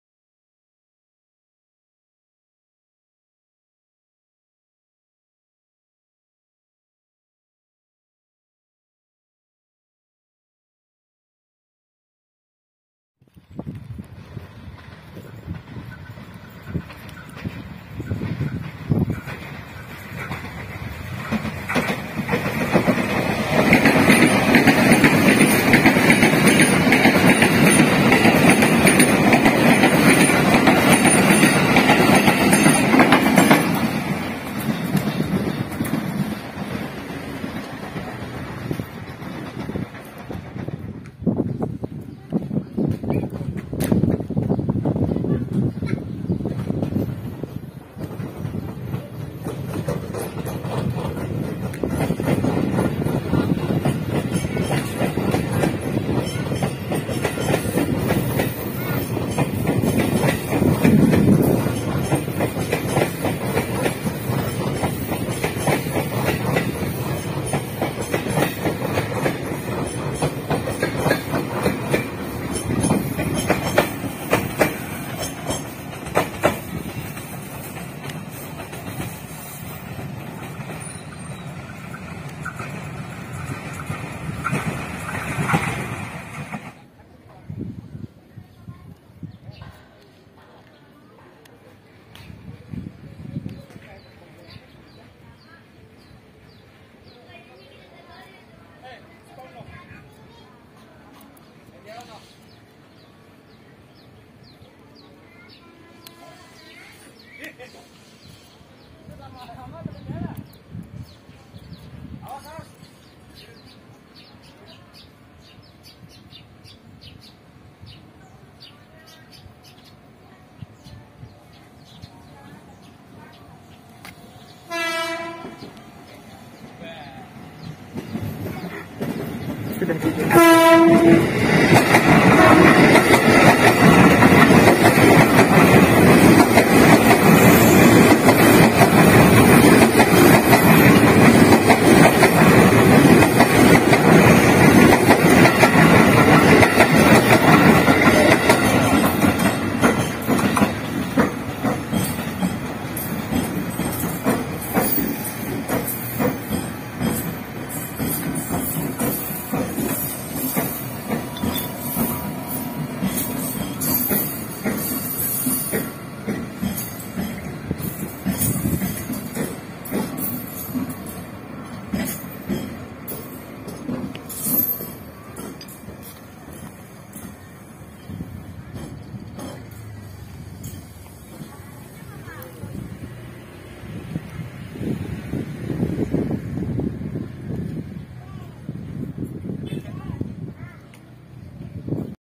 TEST RUN Seri 東京メトロ6000系 6033-6133F dengan Livery Barunya!! 31/1/2022.
Ini adalah video seri Tokyo Metro 6000 yaitu seri 6033F yang baru saja menjalani P48 Di dipo depok sekaligus Mengganti liverynya dengan Livery KAI Commuter yaitu Oren abu-abu. Tapi bisa dilihat dari dekat.. Itu bukan abu-abu melainkan Putih seperti Cat Dasar saja jika dilihat sekilas.. Itu tidak masalah yang penting seri chopper 6000 akan sehat terus ya! Semangat Chopper!!! Thankyou For watching and Enjoy! よろしくお願いします！😊😁。